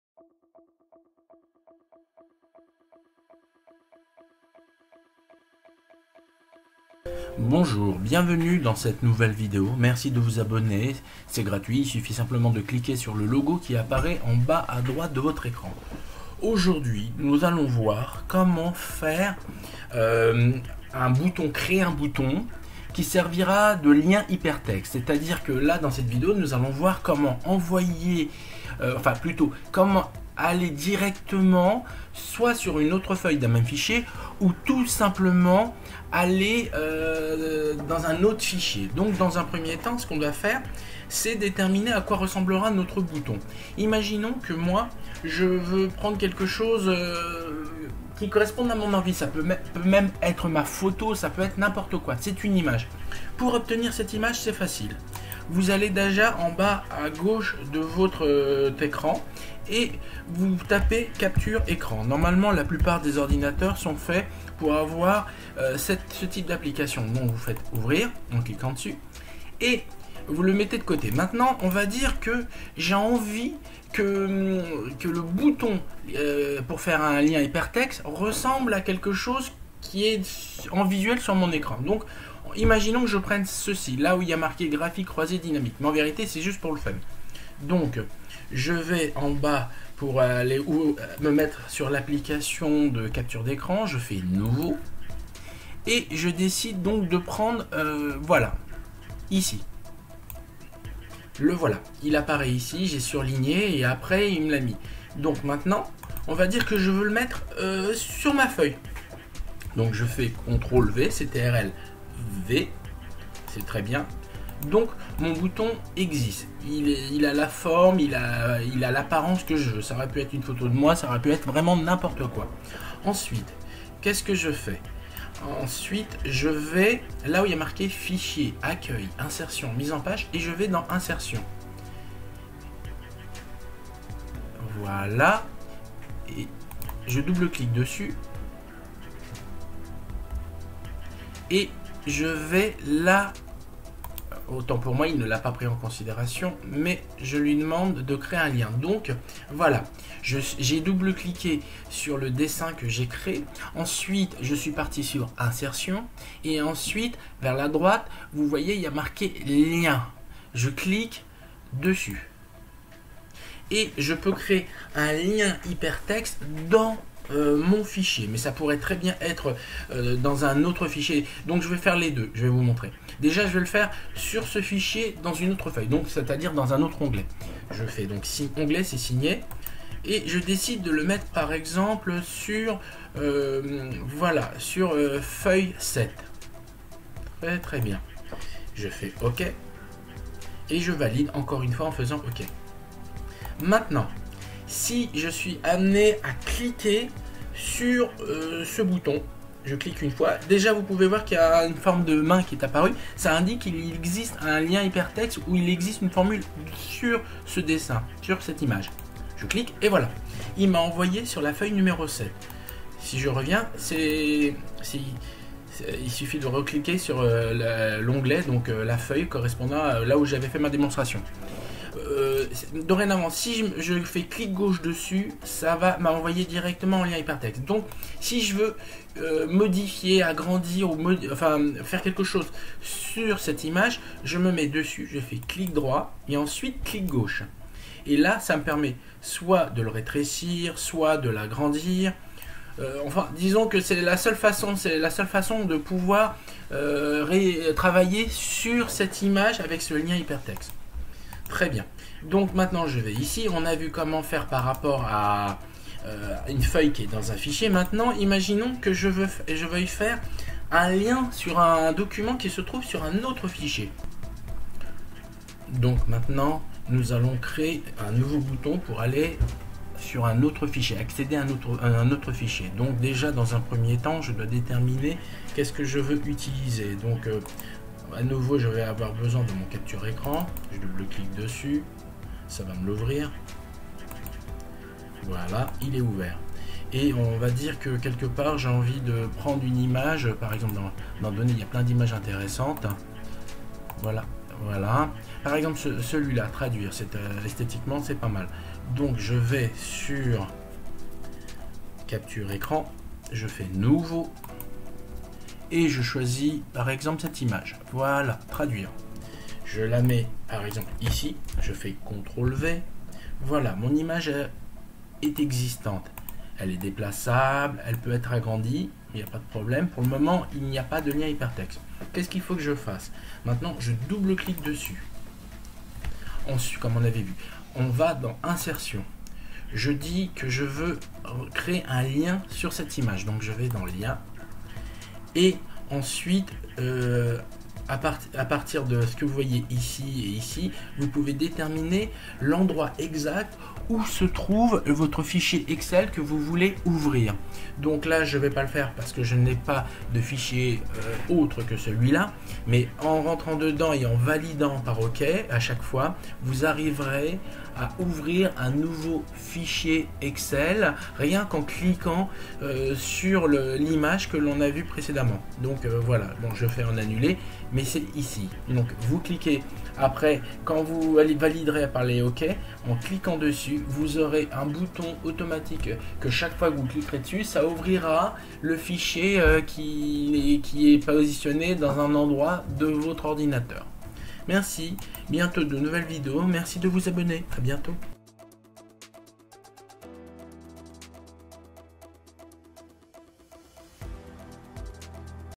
PREMIER PAS SUR EXCEL POUR SUPER DÉBUTANT CREER UN BOUTON POUR LIEN HYPERTEXT
Dans cette vidéo je vais vous montrer comment sur Excel vous pouvez créer des boutons de toutes pièces dans lequel vous pourrez insérer des liens hypertexte cela vous permettra en cliquant sur ce bouton que venez de créer d'aller automatiquement et directement sur le lieu que vous aurez choisi